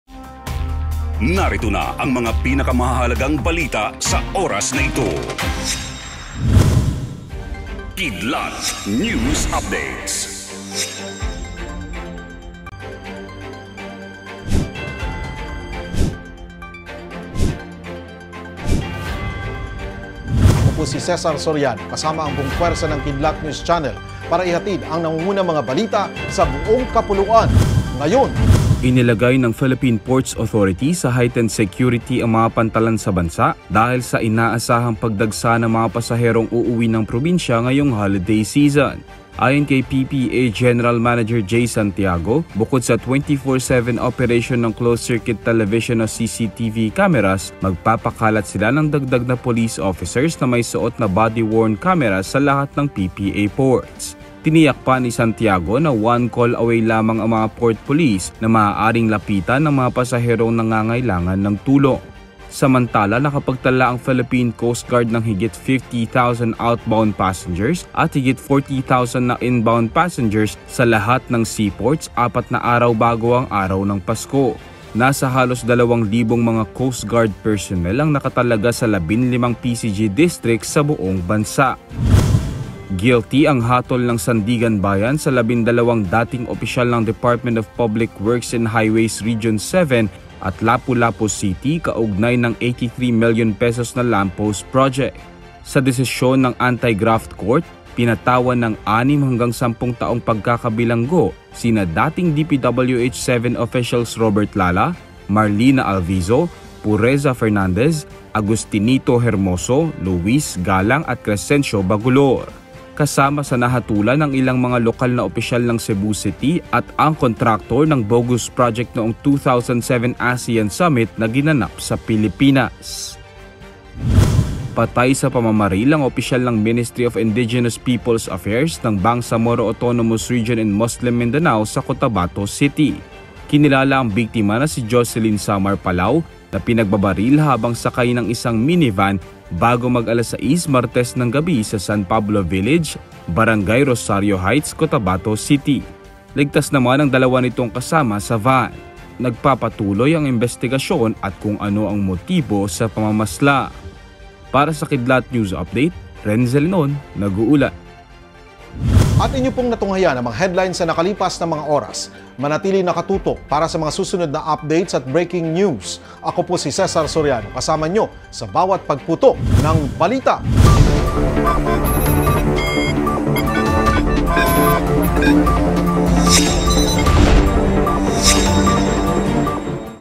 BARMM Official Patay sa Ambush | Kidlat News Update (December 21, 2022 12NN)
Patay sa pamamaril ang opisyal ng Ministry of Indigenous Peoples Affairs  ng Bangsamoro Autonomous Region in Muslim Mindanao sa Cotabatao City.  Kinilala ang biktima na si Jocelyn Samar Palao na pinagbabaril habang sakay ng isang mini van bago mag-alas sais martes ng gabi sa San Pablo Village, Barangay Rosary Heights, Cotabato City. Ligtas naman ang dalawa nitong kasama sa van. Nagpapatuloy ang imbestigasyon at kung ano ang motibo sa pamamaslang.

Ngayon Na!